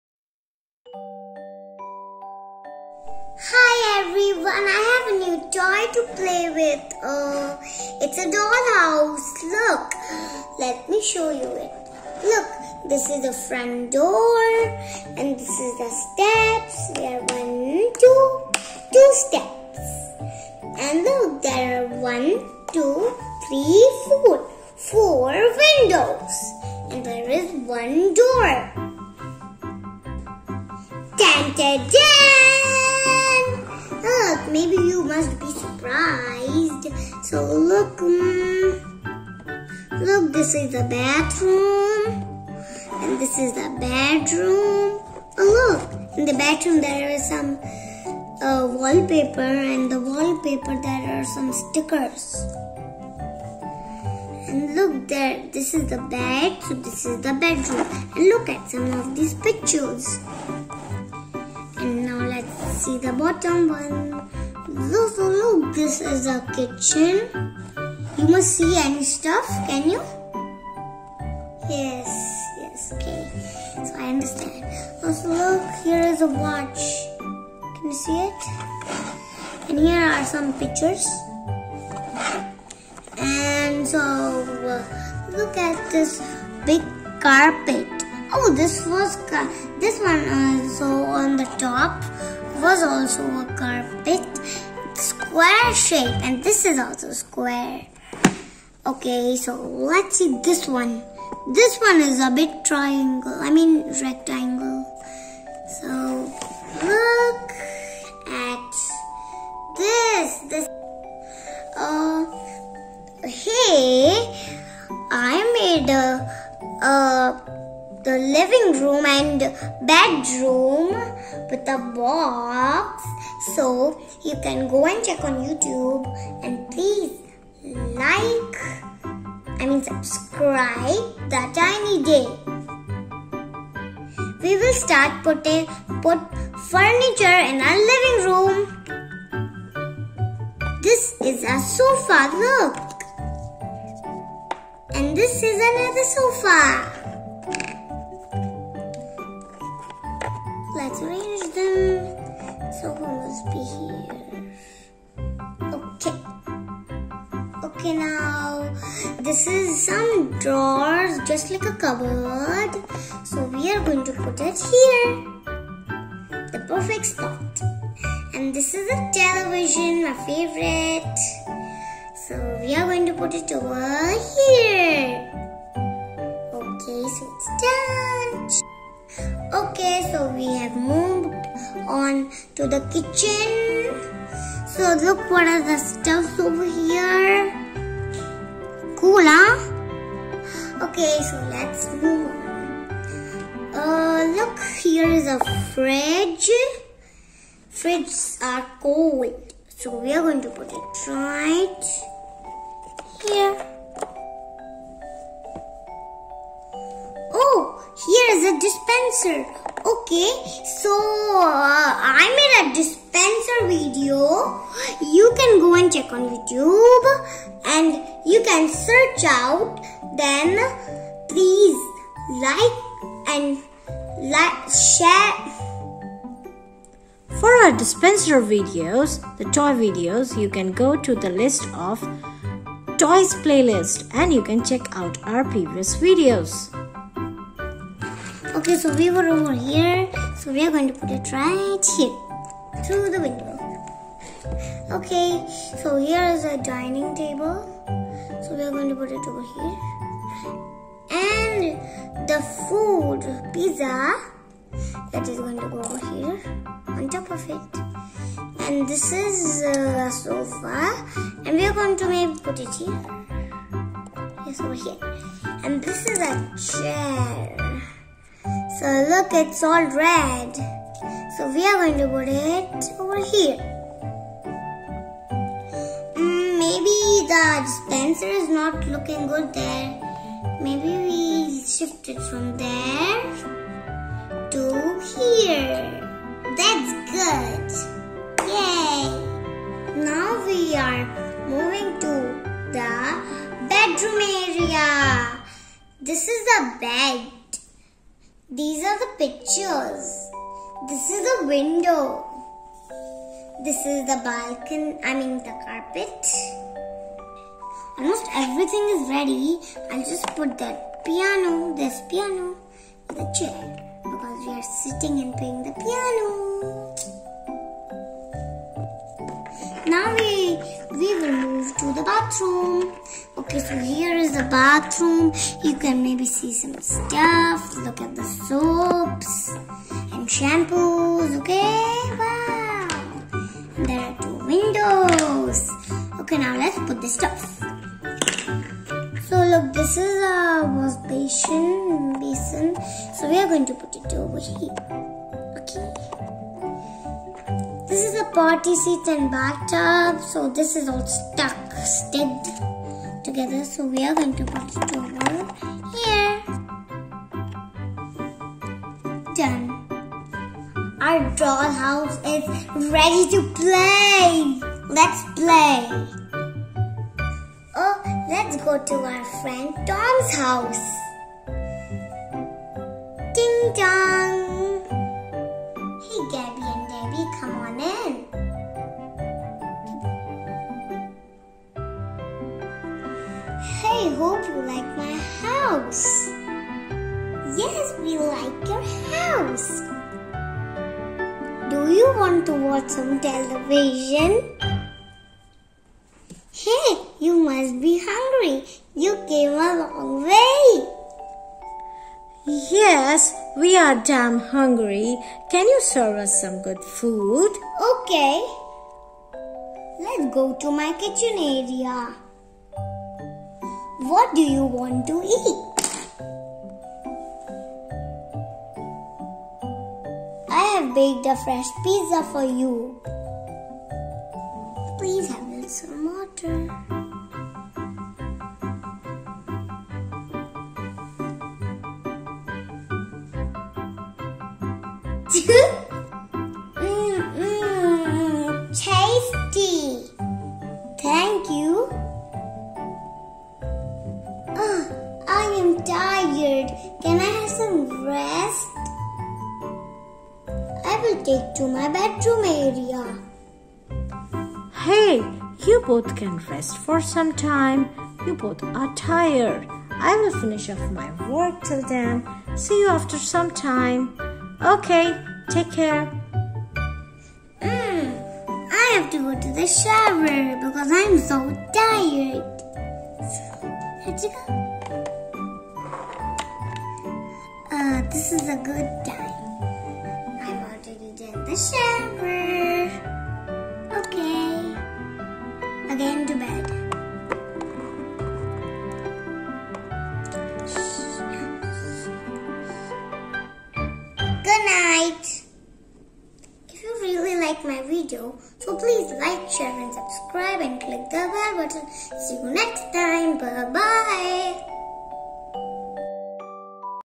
Pretend play Dollhouse | Dollhouse Video
Tinydale family fun video for kids. Kids toys dollhouse unboxing and review. Gabby and Dabby find a special friend. In the video we play with Lego friends. Family-friendly fun for kids of all ages!! Includes a cute pretend play story for children.

Thanks